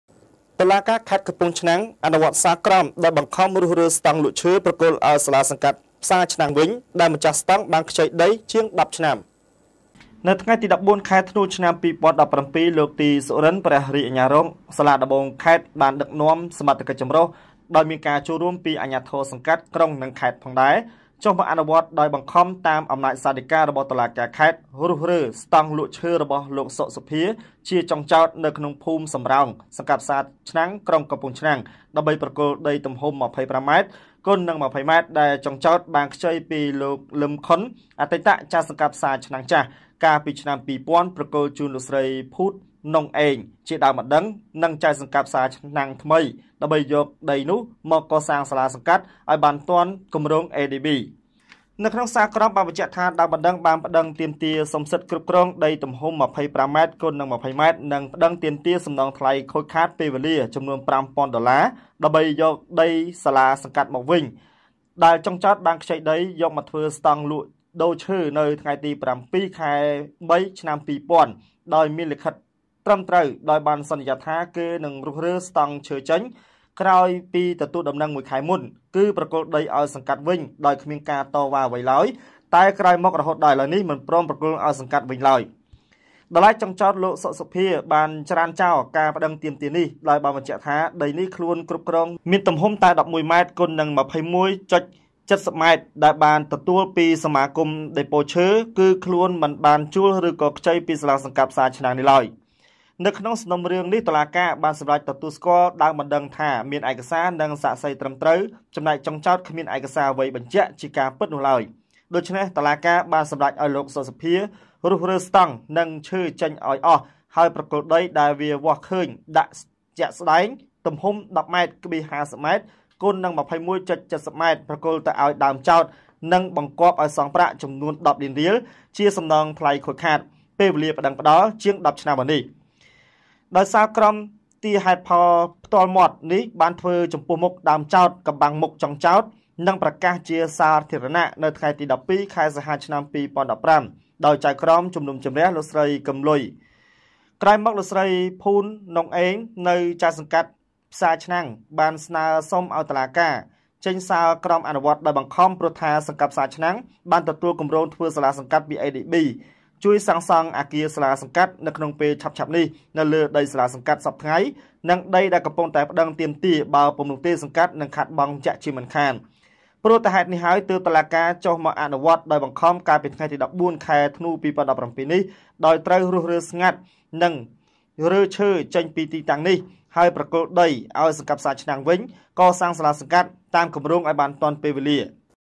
តុលាការខេត្ត អនុវត្តសាលក្រម ដោយបង្ខំរុះរើស្តង់លក់ឈើ ប្រគល់អោយសាលាសង្កាត់ផ្សារឆ្នាំងវិញ
តុលាការខេត្ត អនុវត្តសាលក្រម ដោយបង្ខំរុះរើស្តង់លក់ឈើ ប្រគល់អោយសាលាសង្កាត់ផ្សារឆ្នាំងវិញ ដែលម្ចាស់ស្តង់ បានខ្ចីដីជាង១០ឆ្នាំ